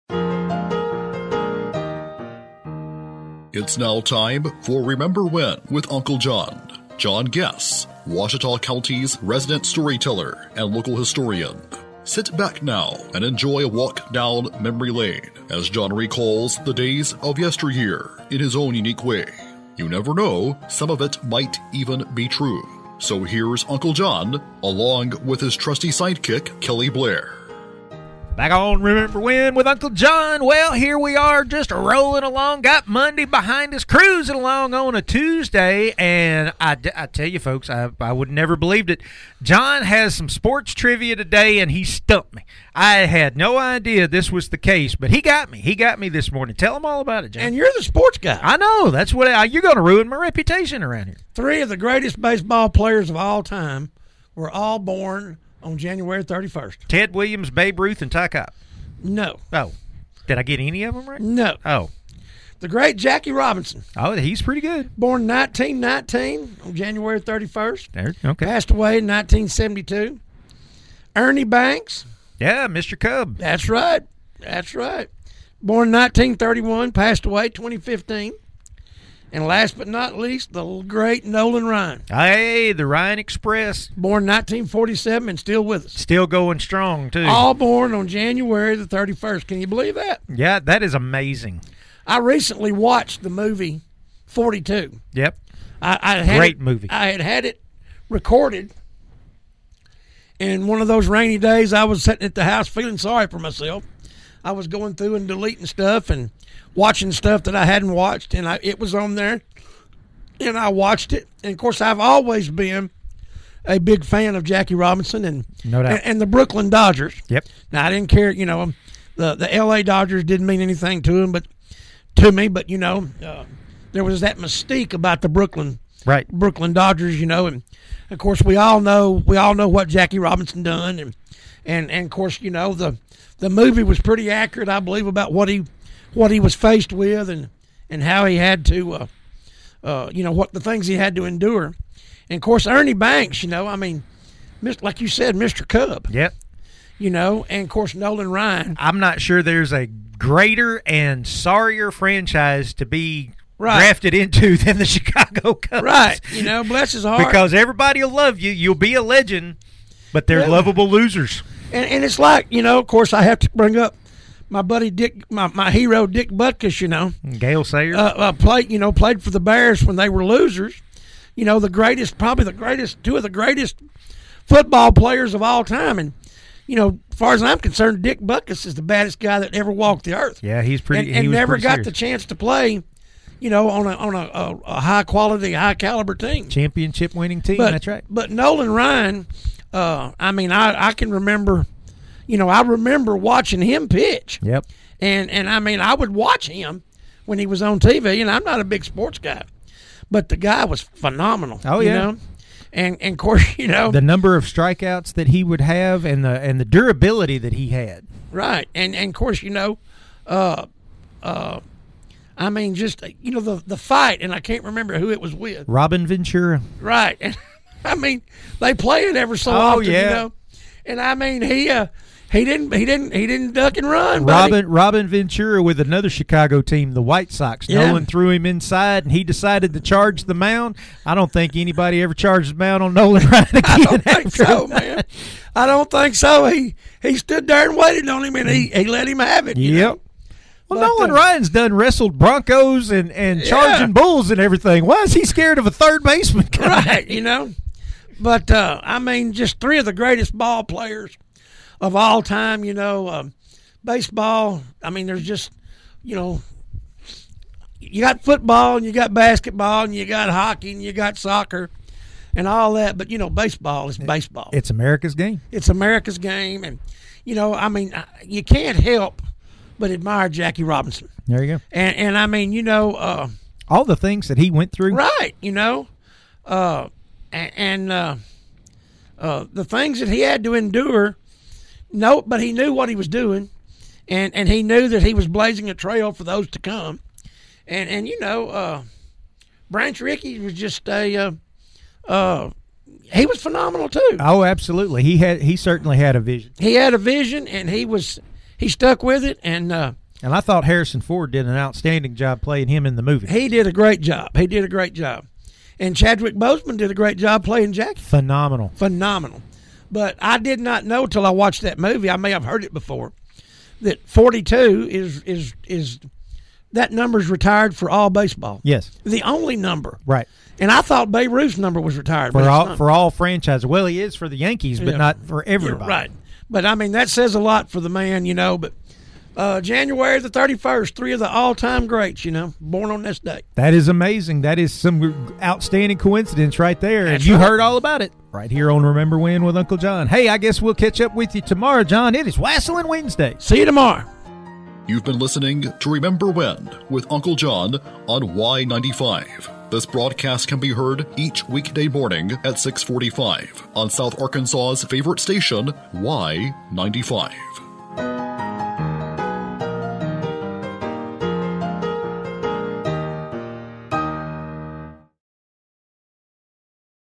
Robinson, Banks, and Ryan
Uncle John recognizes some baseball legends, all of whom were born on the same day (January 31).